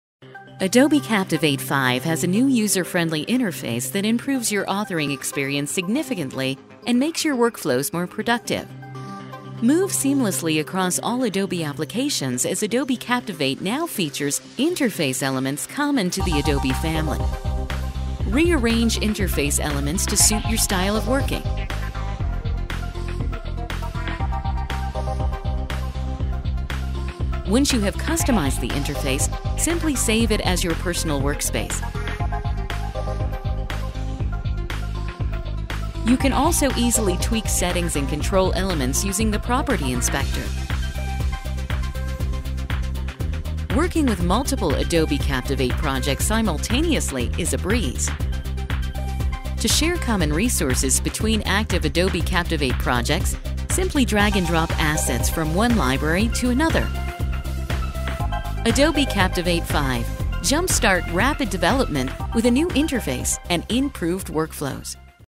Adobe Captivate 5 Optimized User Interface
An overview of the enhanced User Interface in the new version of Adobe Captivate 5. The UI has been modernized to conform with the conventions and aesthetics of other popular Adobe Applications like Adobe Photoshop & Adobe Dreamweaver. This brief overview illustrates the new interface.